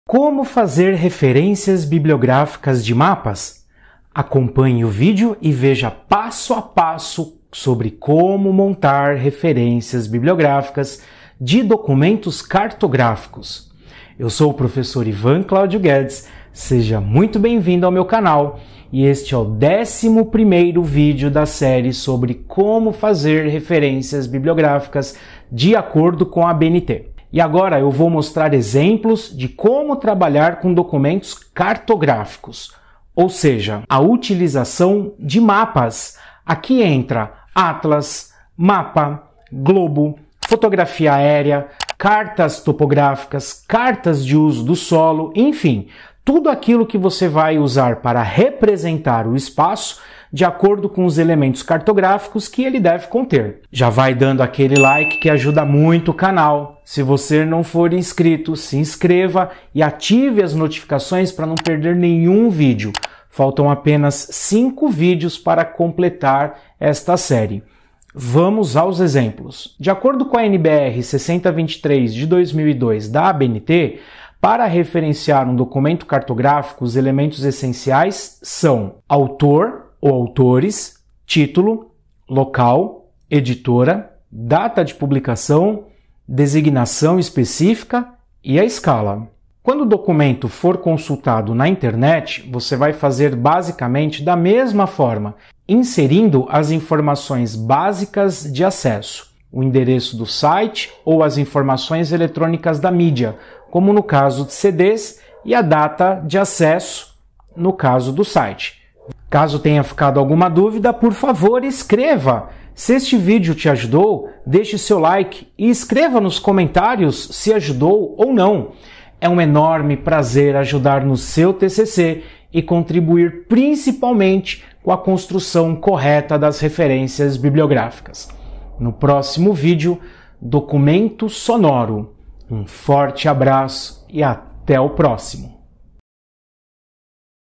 Referências Bibliográficas de mapas 🌐 documentos cartográficos - Vídeo 11/16
Referências bibliográficas abnt 2018. Como fazer referências bibliográficas de mapas, globos, imagens de satélite, ou seja, como fazer referências bibliográficas de documentos cartográficos. 🌎

Seja bem vindo à série sobre como fazer referências bibliográficas de acordo com a ABNT, NBR 6022.

Referência bibliográfica é uma forma de organizar a lista de autores que você utilizou no seu trabalho (TCC, Monografia, Dissertação, Tese, Artigo, Relatórios, Projetos, etc.) de forma com que o leitor possa consultar e localizar efetivamente todas as referências utilizadas.